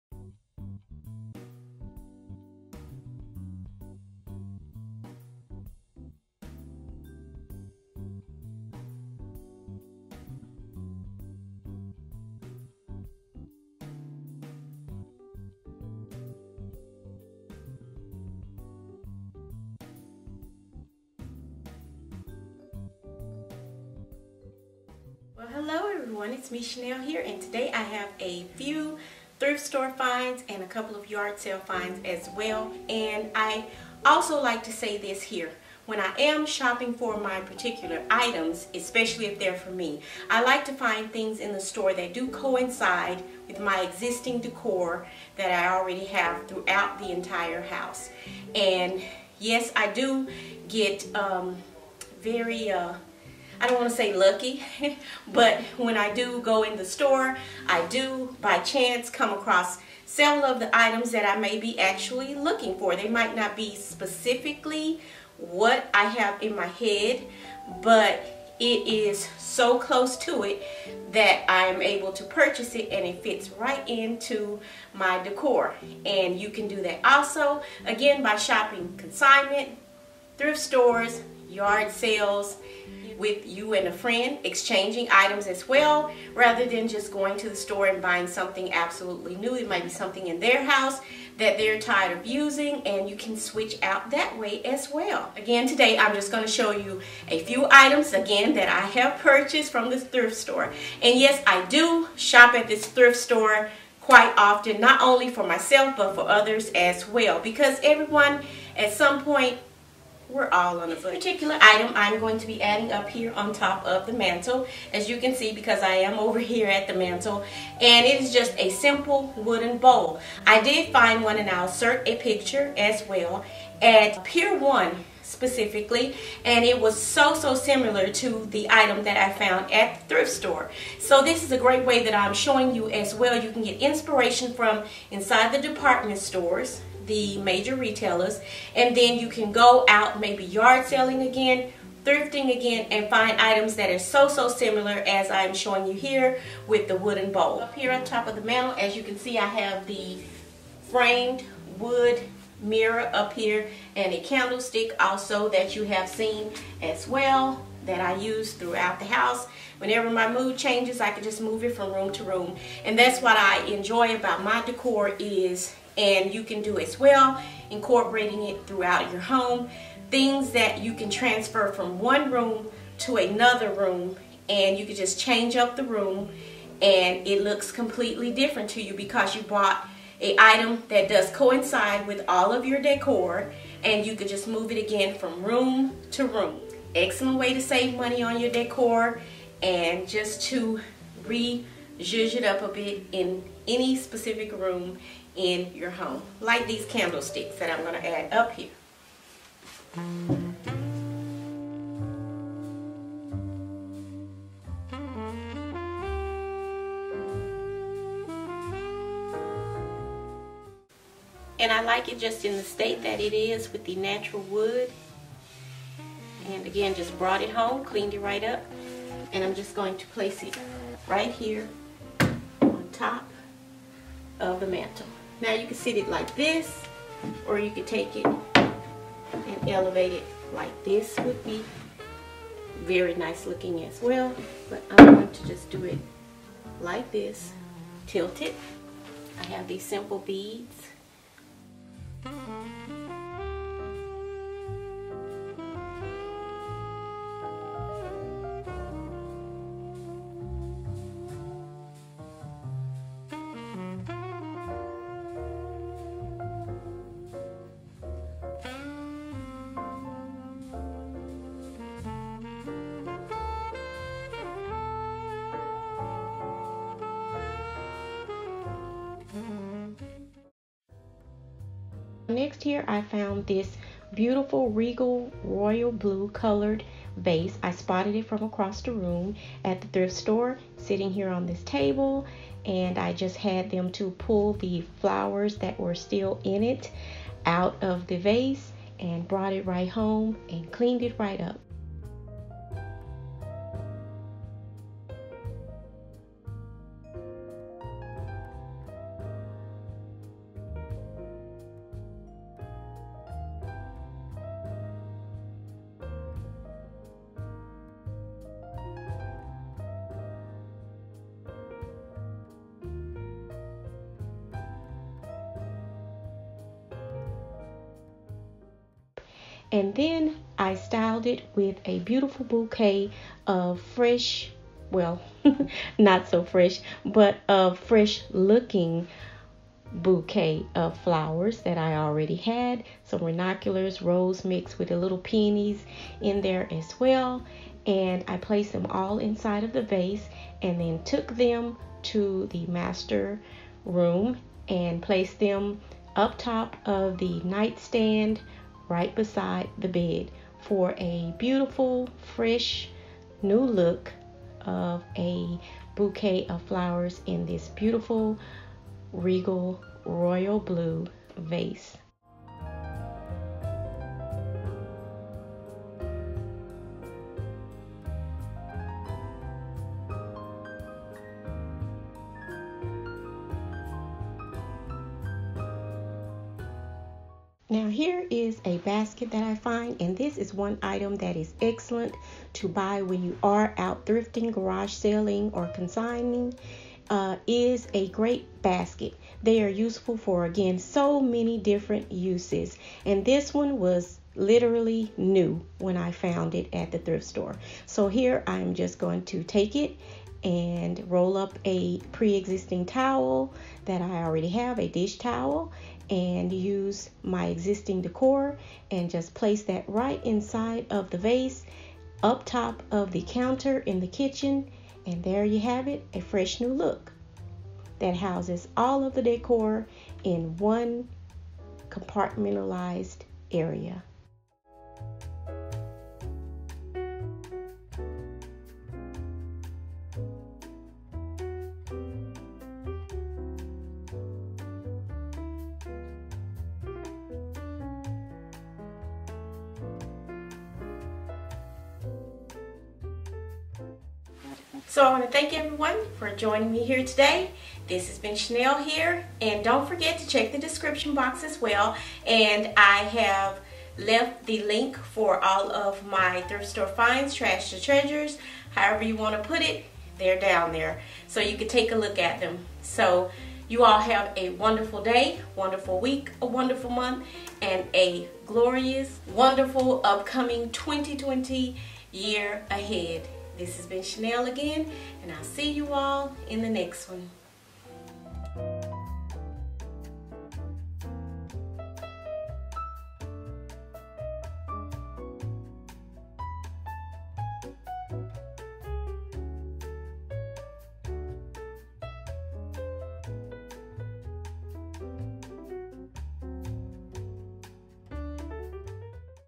Thrift Store Finds 2020
Welcome to my 2020 new thrft store finds trash to treasures. I  Hope you enjoy the video and get some information to use for yourself or someone else in their home or yours.

@Angelic Interior Designs